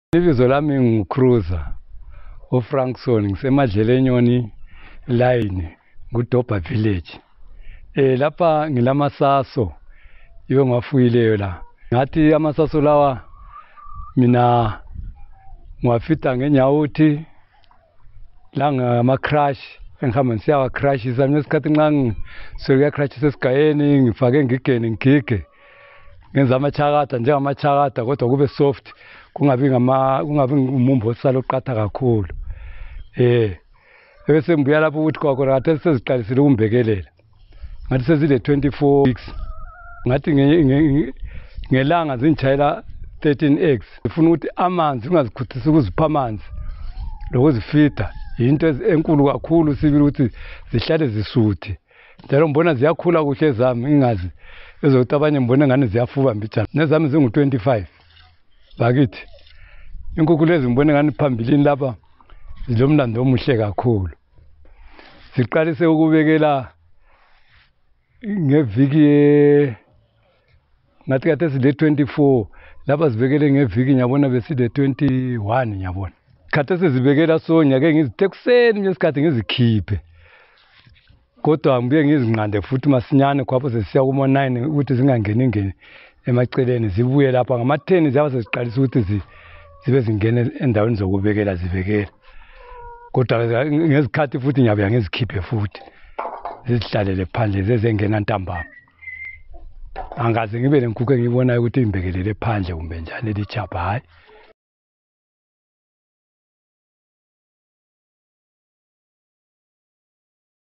Cruzer Francson
Meet Cruzer, a poultry farmer from Ntabazinduna as he takes us through his journey.